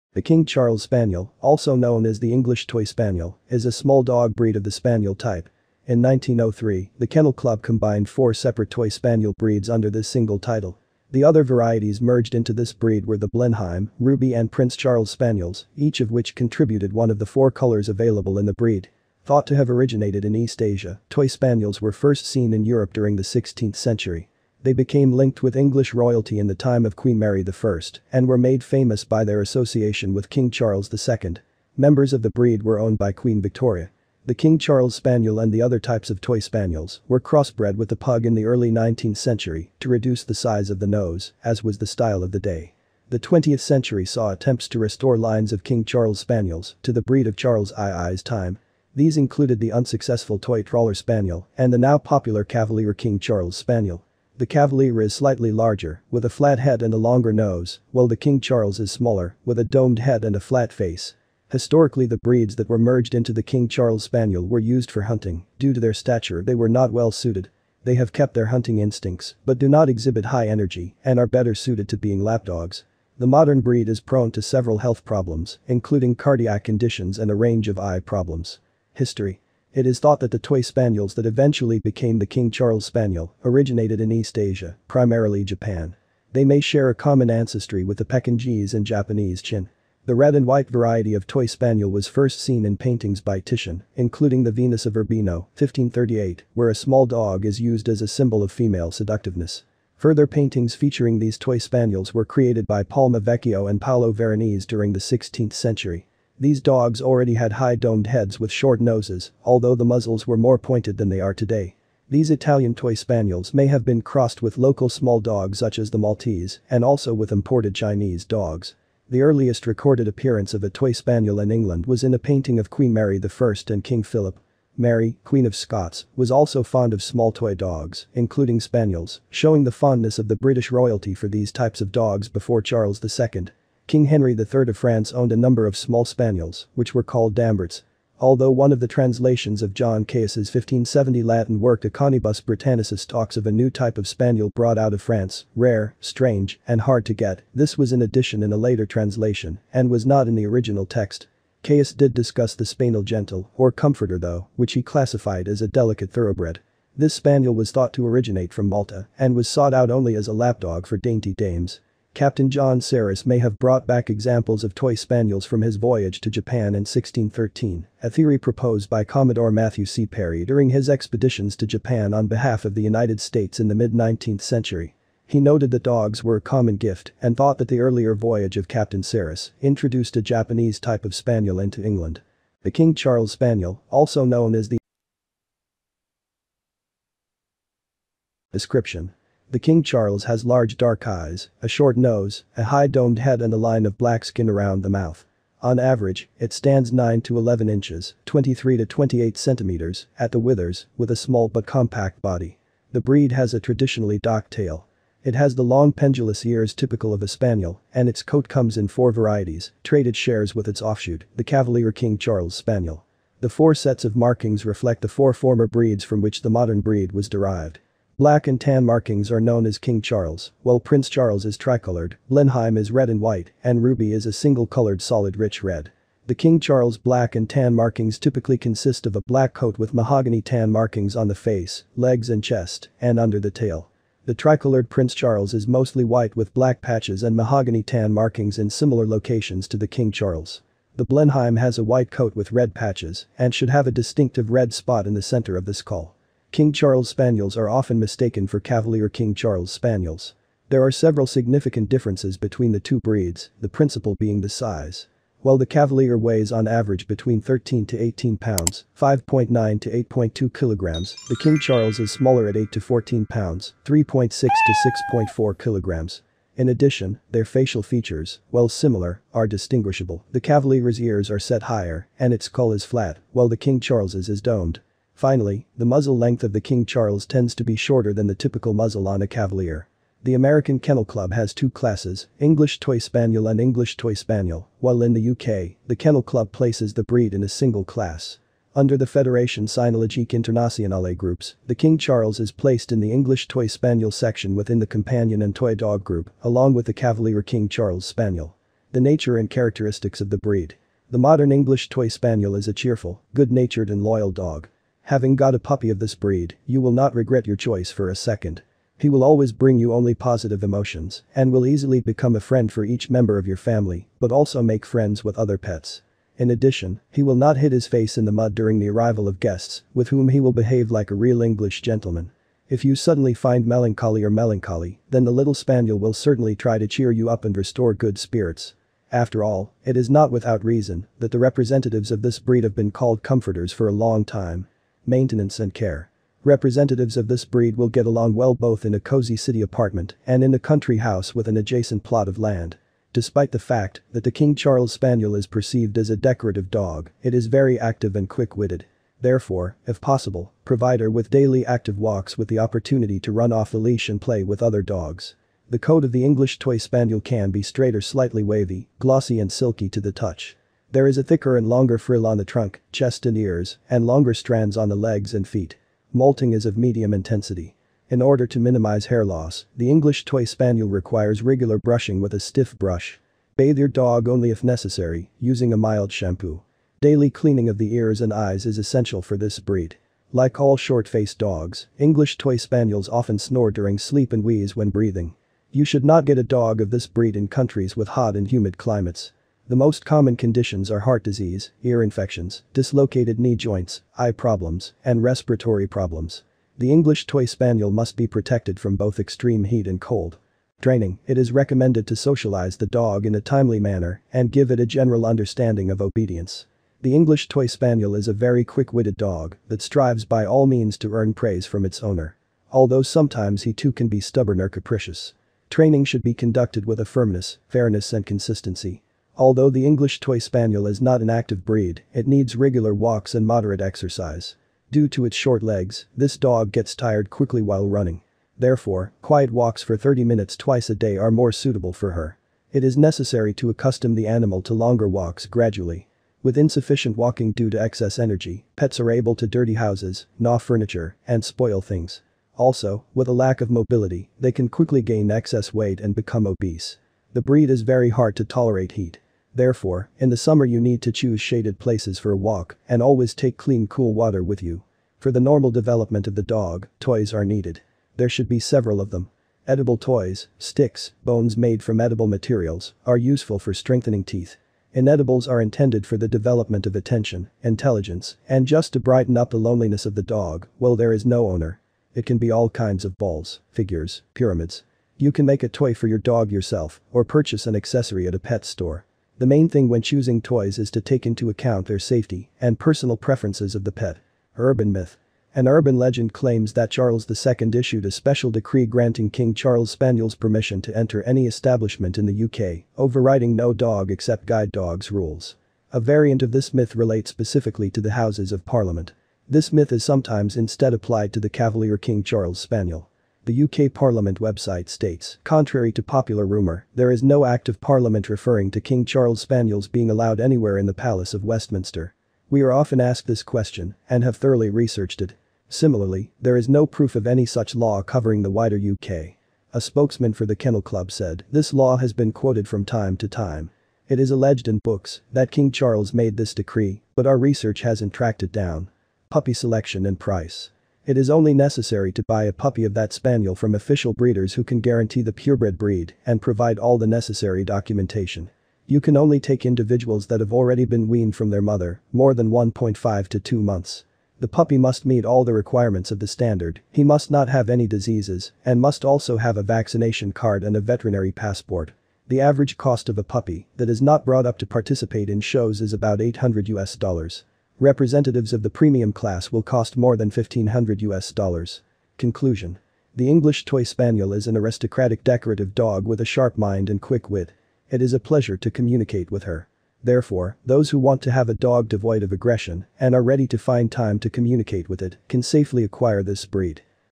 English Toy Spaniel. Pros and Cons, Price, How to choose, Facts, Care, History
The King Charles Spaniel and the other types of toy spaniels were crossbred with the Pug in the early 19th century to reduce the size of the nose, as was the style of the day. The 20th century saw attempts to restore lines of King Charles Spaniels to the breed of Charles II's time. These included the unsuccessful Toy Trawler Spaniel and the now popular Cavalier King Charles Spaniel. The Cavalier is slightly larger, with a flat head and a longer nose, while the King Charles is smaller, with a domed head and a flat face.

Thank you so much !  It HELPS a lot.